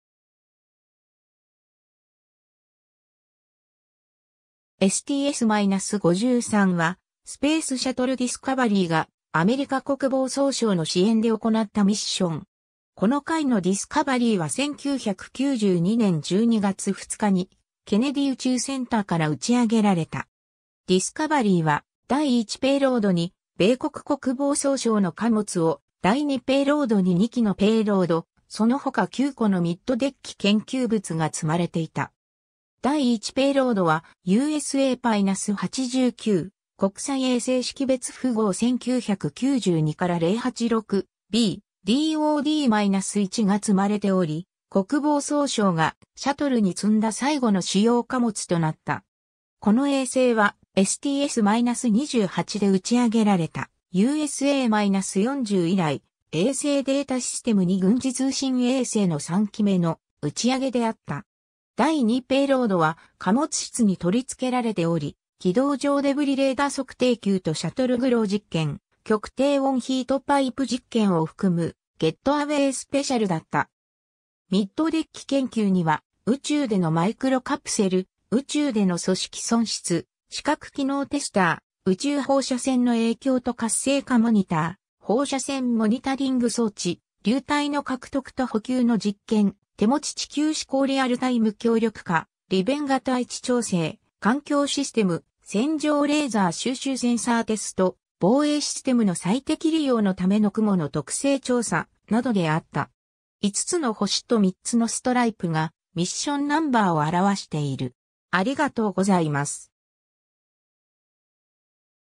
STS-53
STS-53はスペースシャトルディスカバリーがアメリカ国防総省の支援で行ったミッション。
この回のディスカバリーは1992年12月2日にケネディー宇宙センターから打ち上げられた。
 ディスカバリーは第一ペイロードに米国国防総省の貨物を、第二ペイロードに2基のペイロード、そのほか9個のミッドデッキ研究物が積まれていた。
 第一ペイロードはUSA-89、国際衛星識別符号1992-086B、"DoD-1"が積まれており、国防総省がシャトルに積んだ最後の主要貨物となった。
 この衛星はSTS-28出打ち上げられたUSA-40以来、衛星データシステム2軍事通信衛星の3基目の打ち上げであった。
 第二ペイロードは貨物室に取り付けられており軌道上デブリレーダー測定球(ODERACS)とシャトルグロー実験/極低温ヒートパイプ実験(GCP)を含むゲット・アウェイ・スペシャル(GAS)だった。
 ミッドデッキ研究には宇宙でのマイクロカプセル(MIS-l)、宇宙での組織損失(STL)、視覚機能テスター(VFT-2)、宇宙放射線の影響と活性化モニター(CREAM)、放射線モニタリング装置(RME-III)、流体の獲得と補給の実験(FARE)、手持ち地球指向リアルタイム協力下利便型位置調整・環境システム(デジタルカメラ利用実験、HERCULES)、戦場レーザー収集センサーテスト(BLAST)、防衛システムの最適利用のための雲の特性調査(CLOUDS)などであった。
 5つの星と3つのストライプがミッションナンバーを表している。